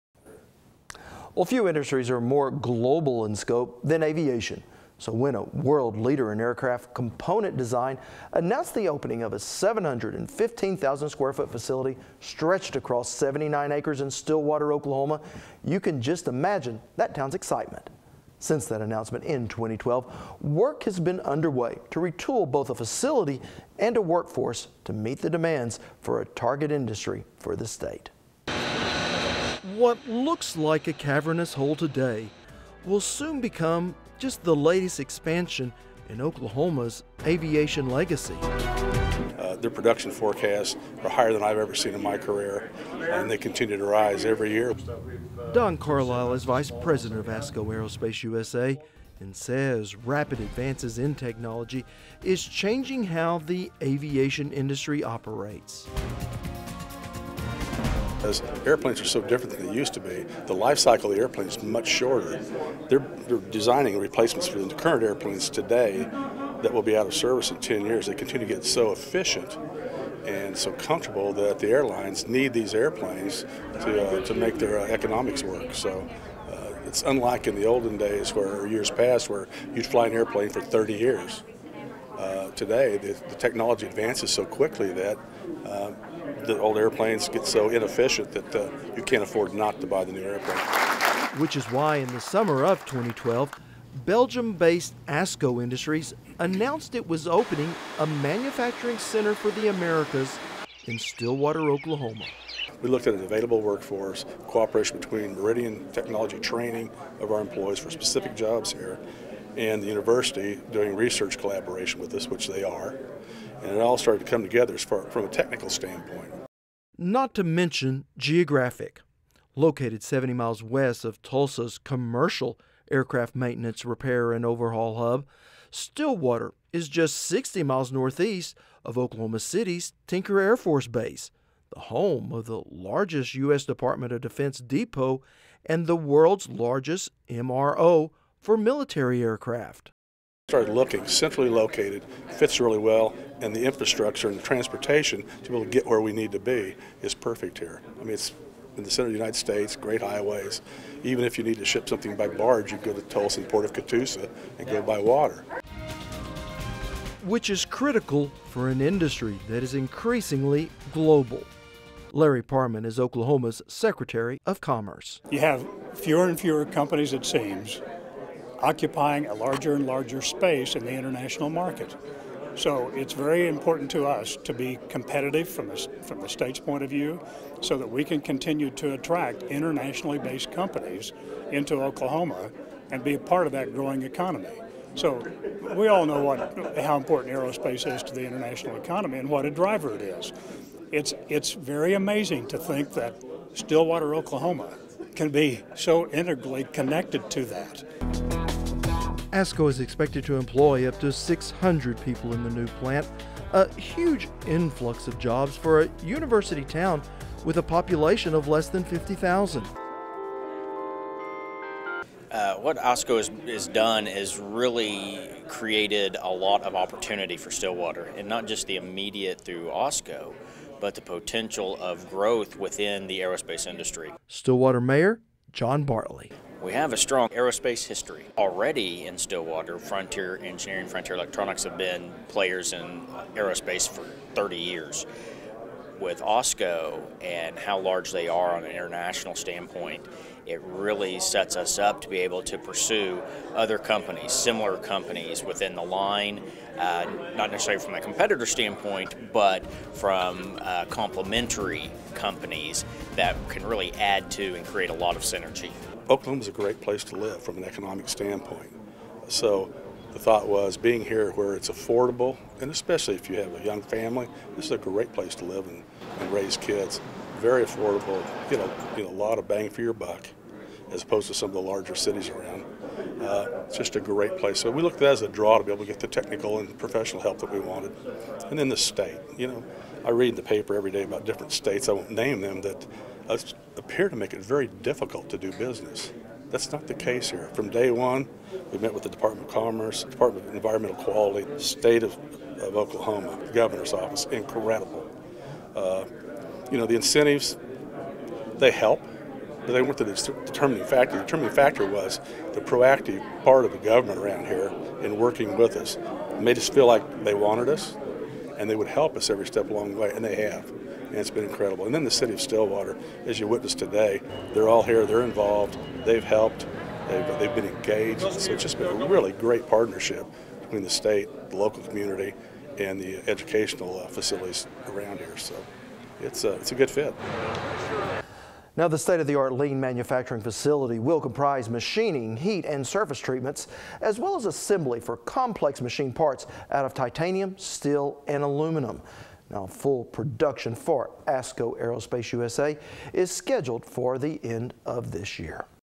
One Stop Shop
ASCO Industries is a world leader in design and manufacture of complex mechanical assemblies for the aerospace industry.